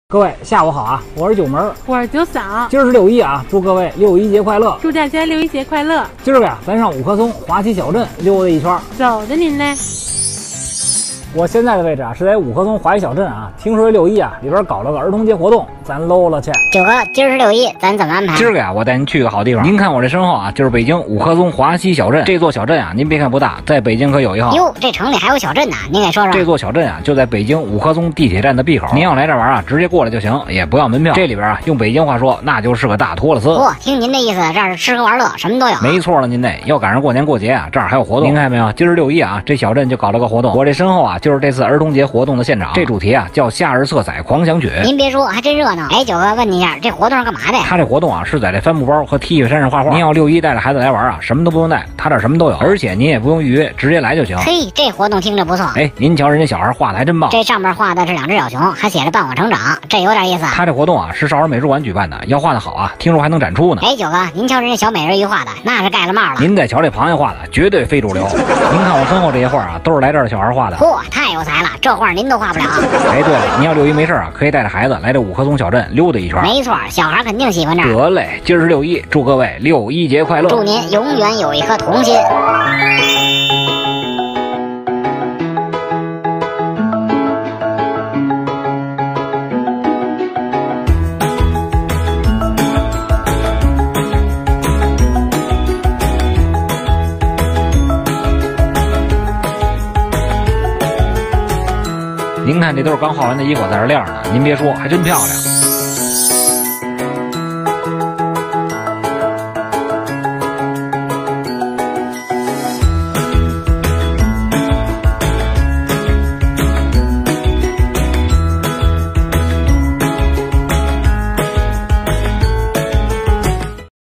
北京六一儿童节去哪玩？五棵松迷你小镇来看看，简直是小孩的乐园 | 九门儿
用一个北京人的视角和记忆～带您发现不一样的北京城！

Following a native’s view and memory of Beijing~to discover different Beijing city!